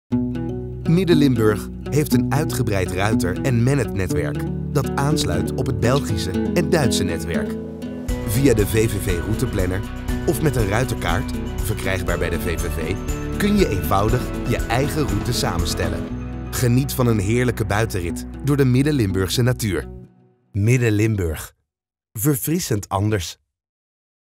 Paardrijden in Midden-Limburg | VVV Midden-Limburg
Midden-Limburg heeft een uitgebreid ruiter- en mennetwerk dat aansluit op het Belgische en Duitse netwerk. Via de VVV routeplanner of met een  ruiterkaart, verkrijgbaar bij de VVV, kun je eenvoudig je eigen route samenstellen. Geniet van een heerlijke buitenrit door de middenlimburgse natuur.